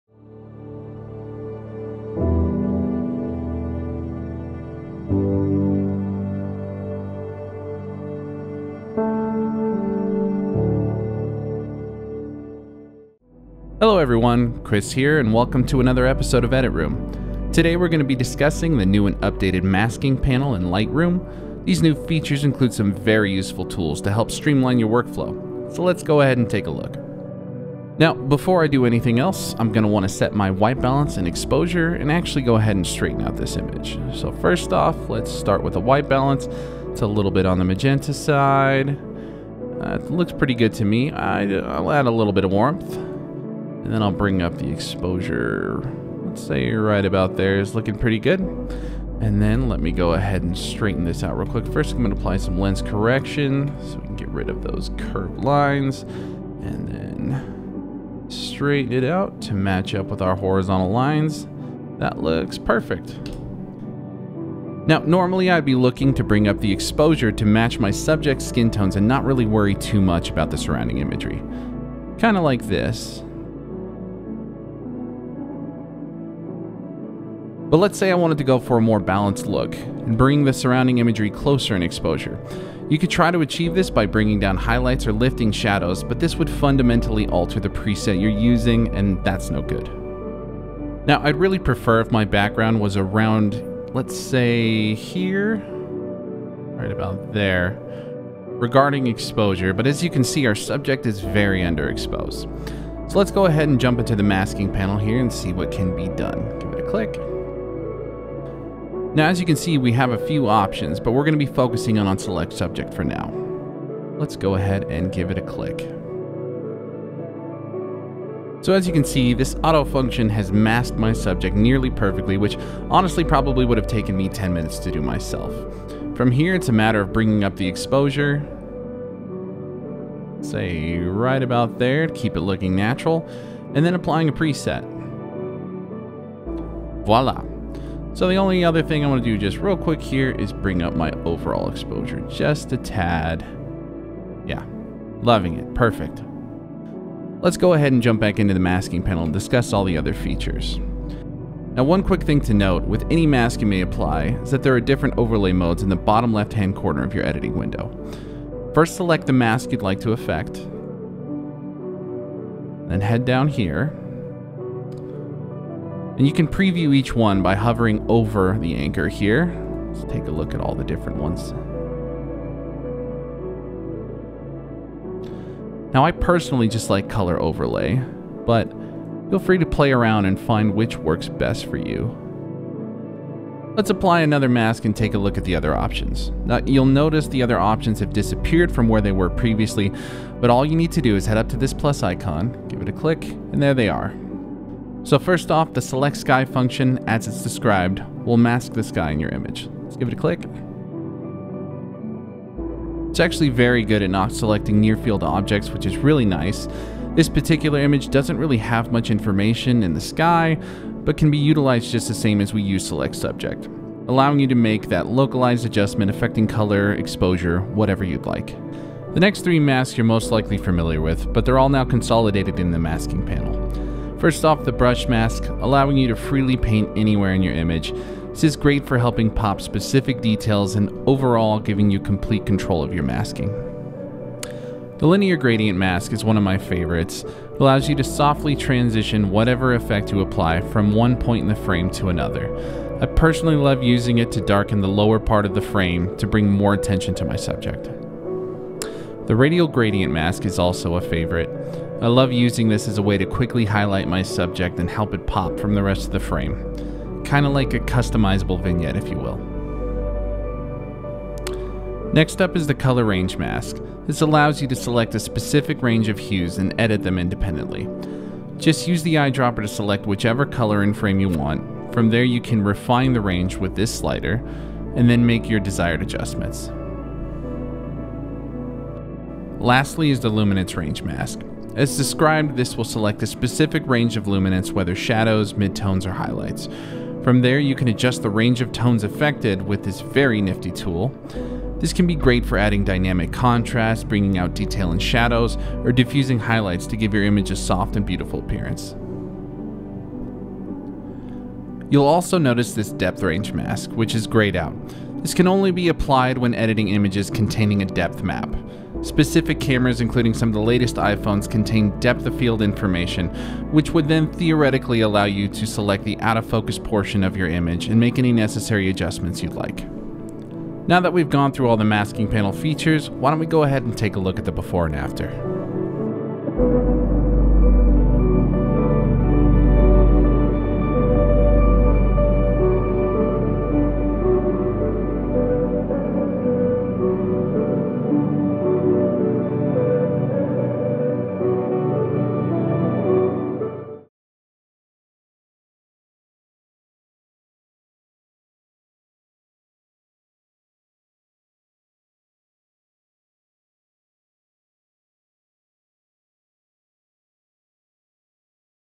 Editroom - Masking Panel
Join Chris and explore Lightroom’s new masking panel features!